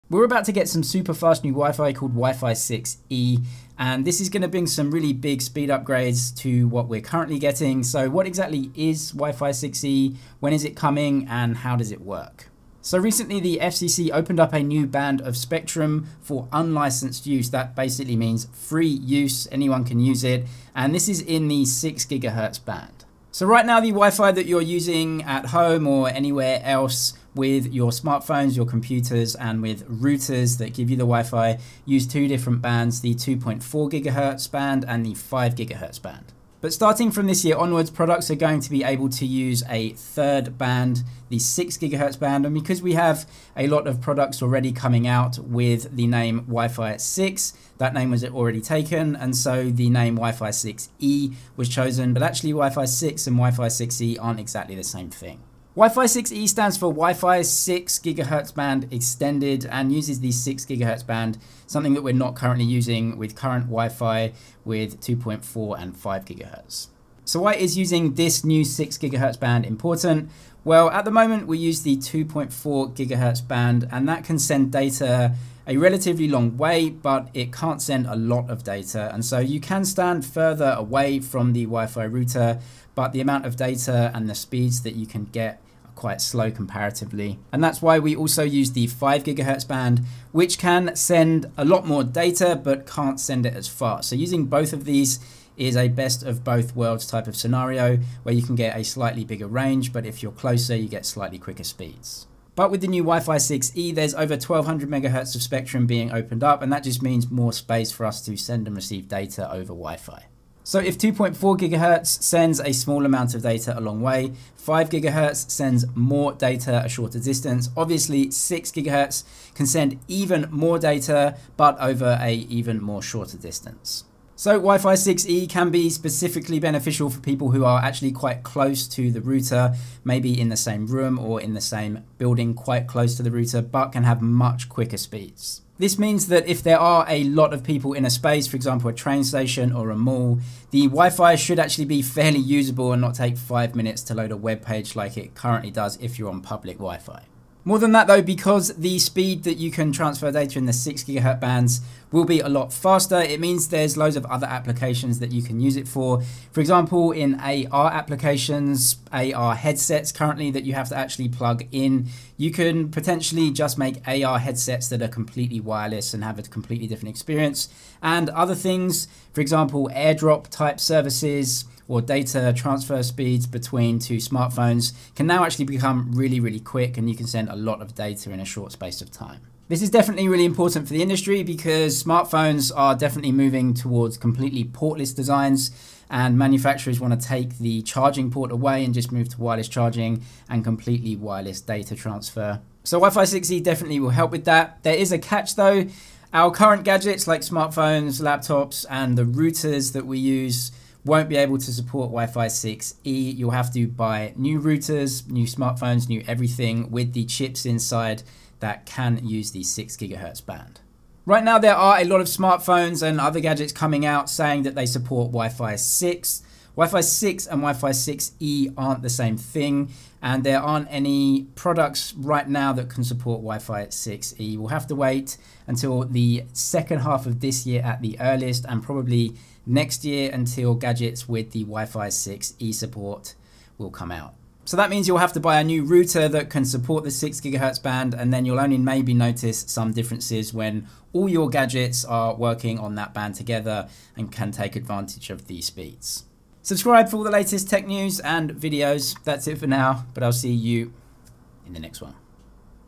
What Is WiFi 6E? Super Fast New WiFi
What is WiFi 6E, how does WiFi 6E work and when will it come to devices?

Smartphone case
Screen protector
Main camera
2nd camera